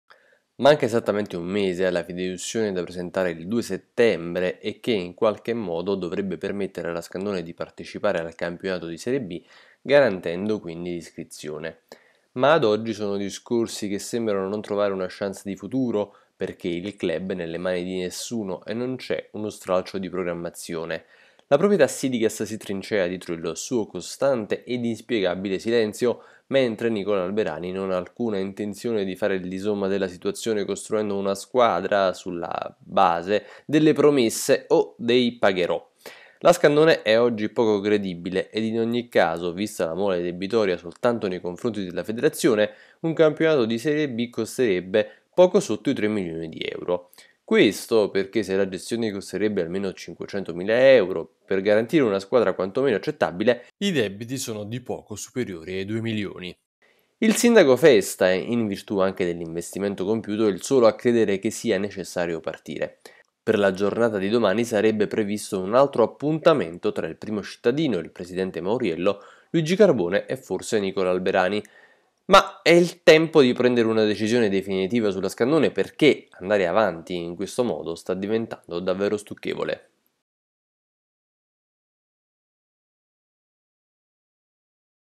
Scandone tutto tace, a crederci è solo Festa
La situazione in casa Scandone Avellino sta diventando davvero stucchevole, tanti silenzi ed i debiti dietro l'angolo pronti a soffocare il club